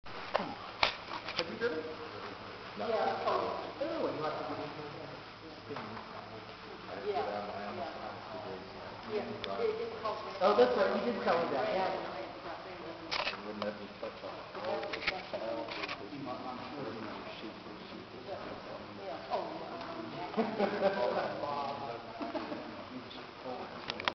Pam and Sundance Driving #1
Pam and Sun John's Grey image driving at Fallen Oak Farm Culpeper Va.on Mothers Day 05/10/2009